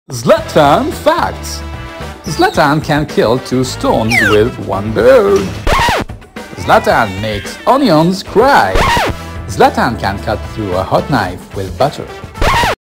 Zlatan Ibrahimovic facts 01
Zlatan facts:
1.      Zlatan can kill two stones with one bird
2.      Zlatan makes onions cry
3.      Zlatan can cut through a hot knife with butter